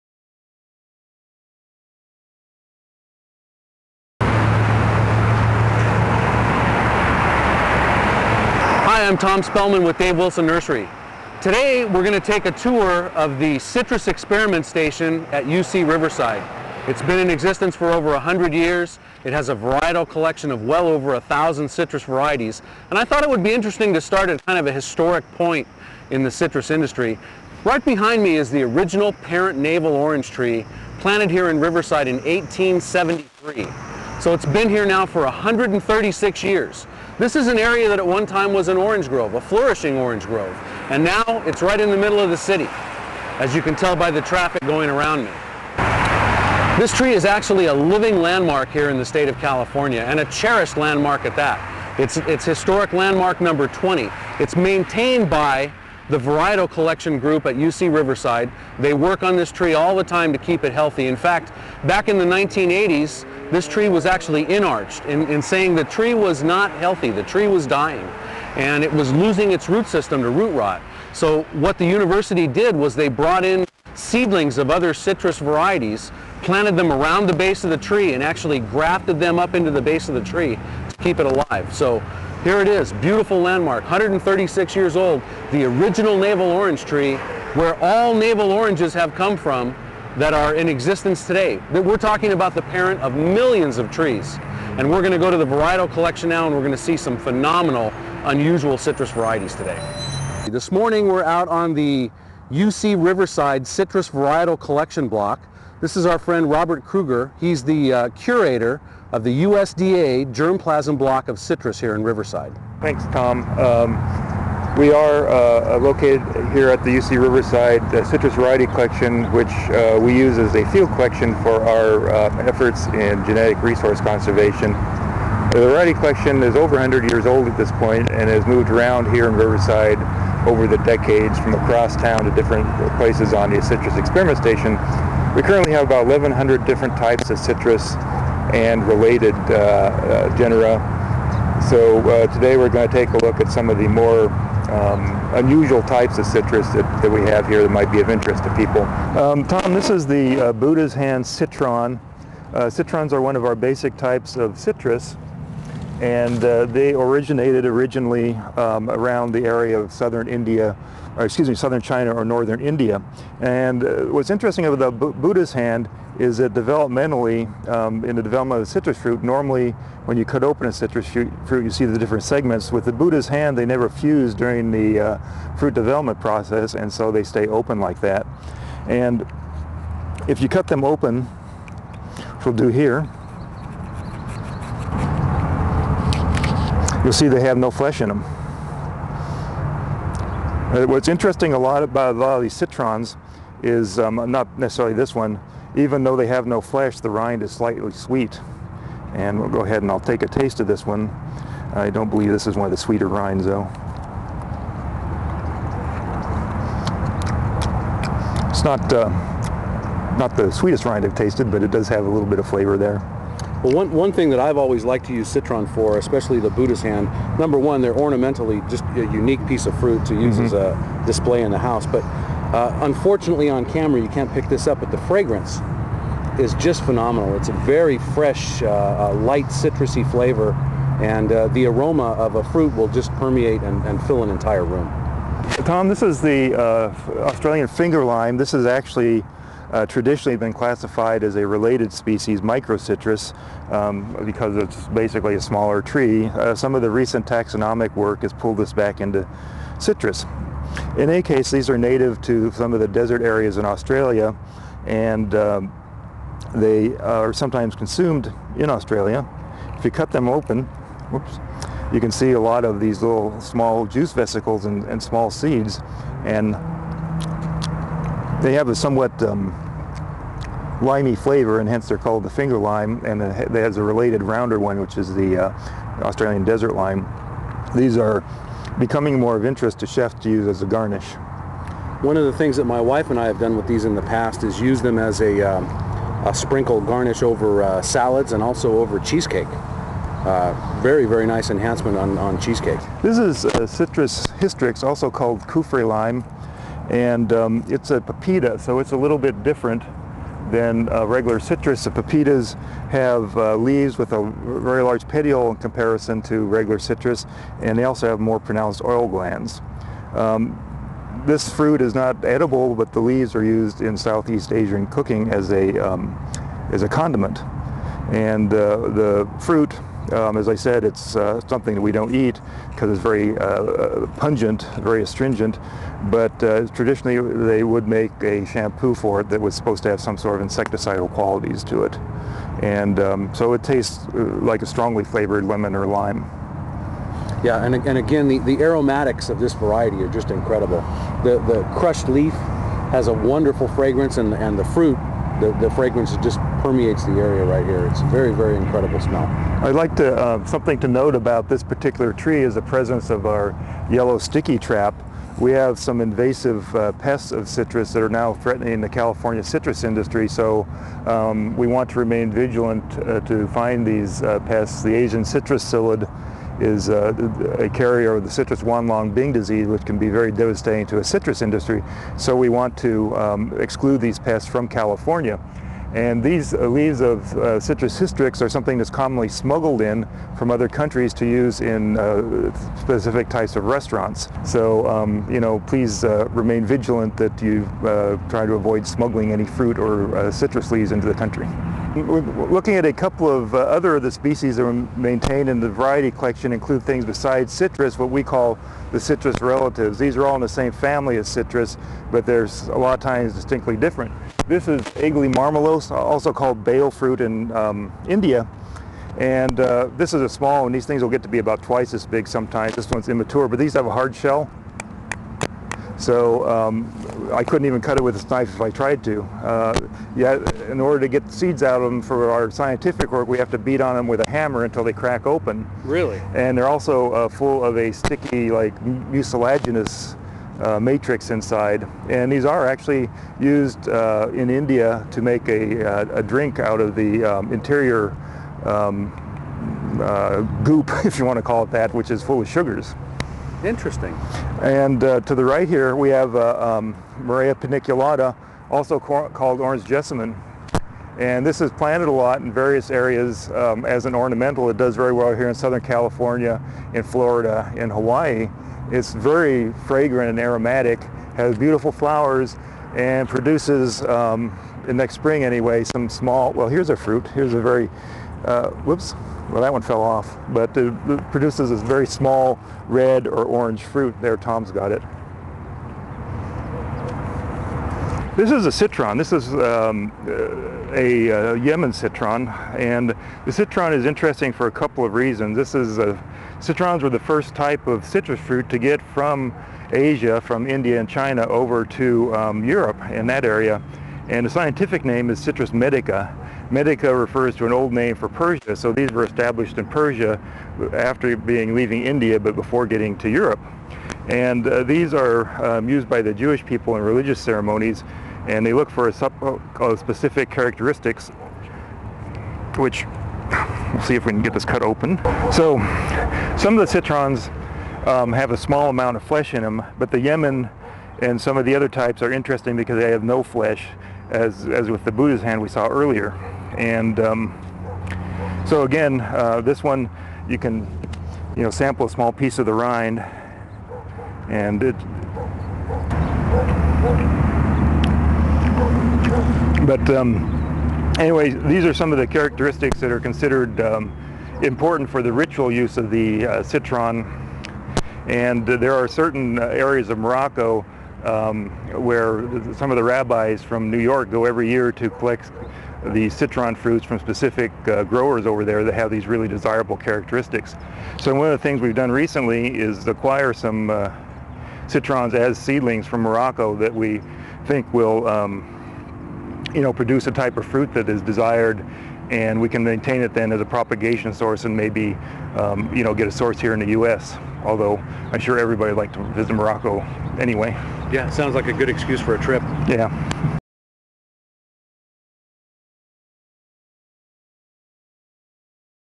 UC RIverside Citrus Collection
A tour of the UC Riverside Citrus Research Station, concentrating on the more unusual citrus varieties.